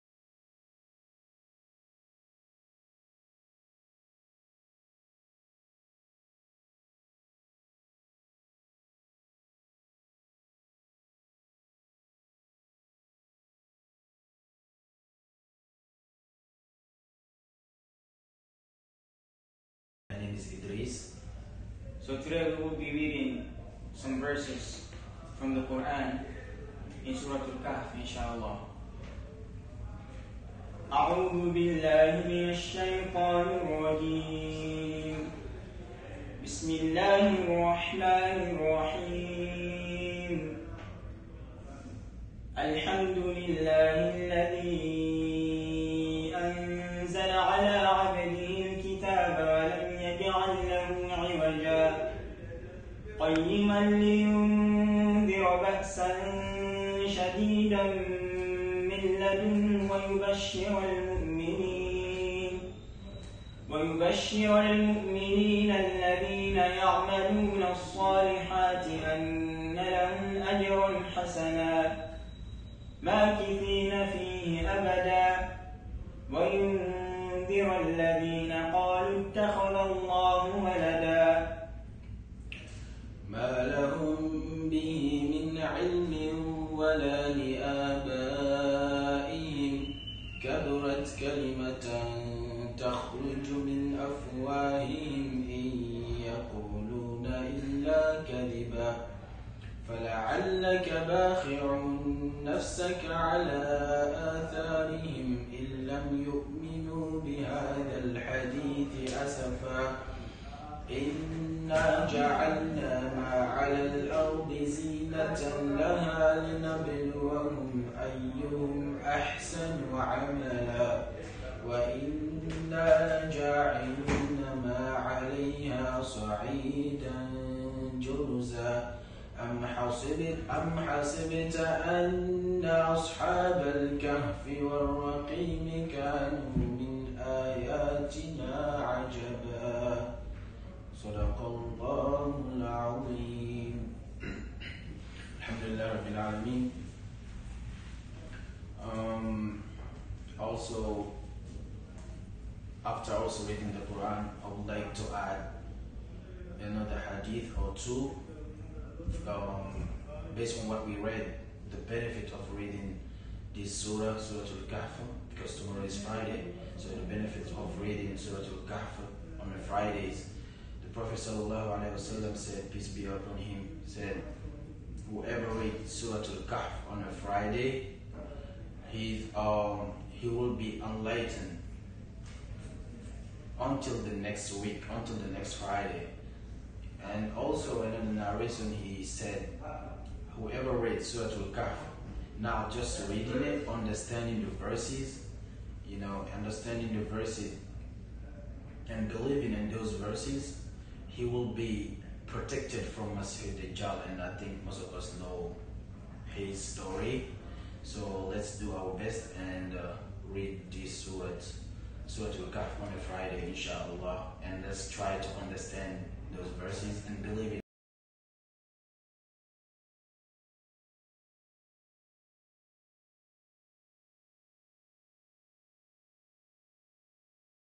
Quran Recitation
Two members of our group reciting verses from surah Al-Kahf (The Cave) and giving the importance of why it is good to read and understand the surah every Jummah.